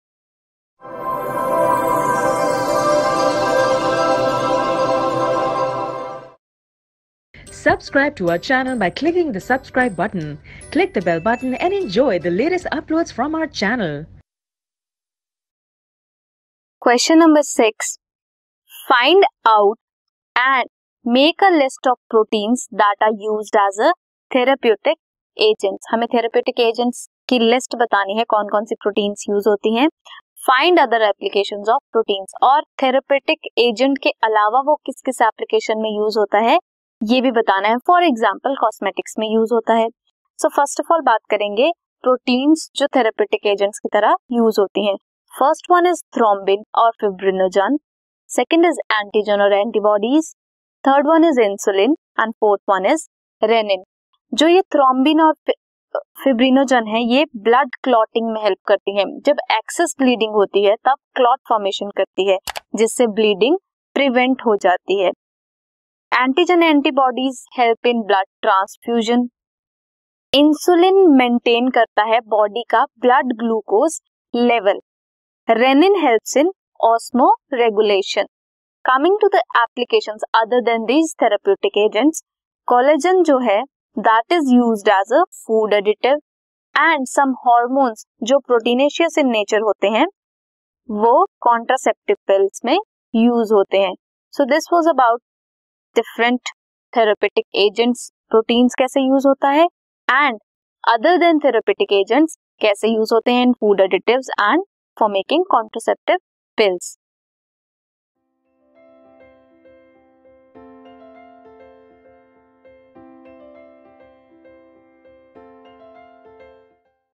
Q6 List proteins used as therapeutic agents and their other applications. - CBSE Class 11 Biology
This video is a part of the series for CBSE Class 11, Biology demo videos for the chapter “Biomolecules”. In this series, we have completed all the concepts, examples and the entire NCERT exercises. It is the part of an initiative taken to provide quality online education to students free of cost on YouTube.